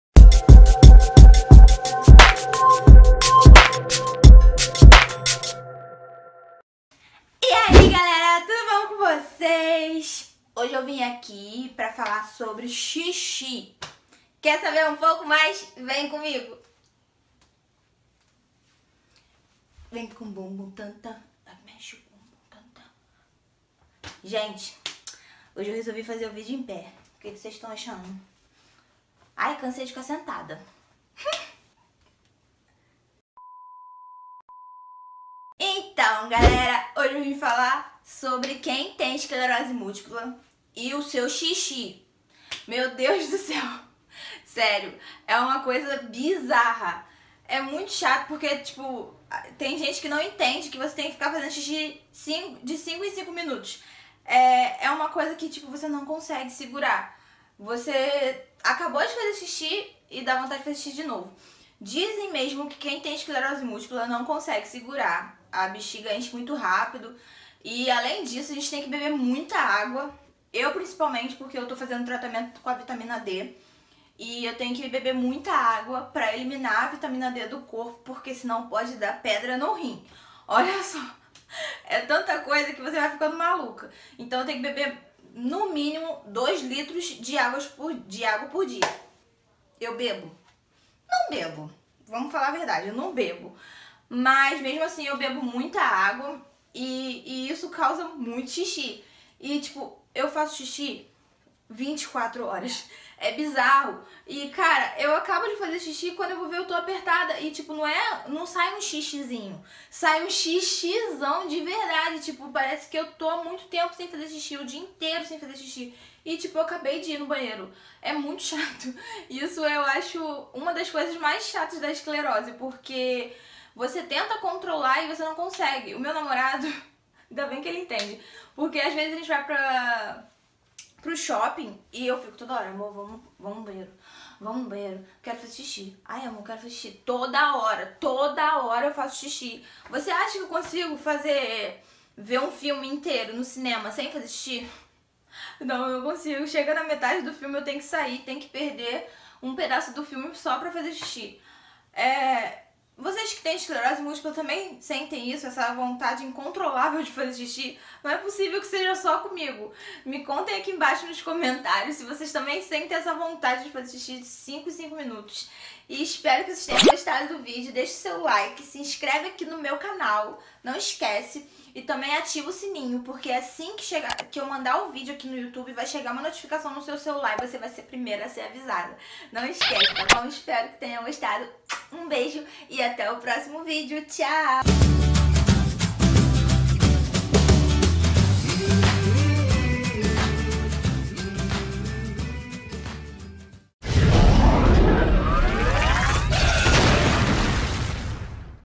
A ESCLEROSE MÚLTIPLA E O XIXI | Nathalia Vieira
Hoje vim falar um pouco sobre o tanto de xixi que faço diariamente por causa da esclerose múltipla. Você que também tem esclerose, sente a mesma coisa? Me conta aqui embaixo nos comentários! Beijos

Créditos finais -

Beijinhos.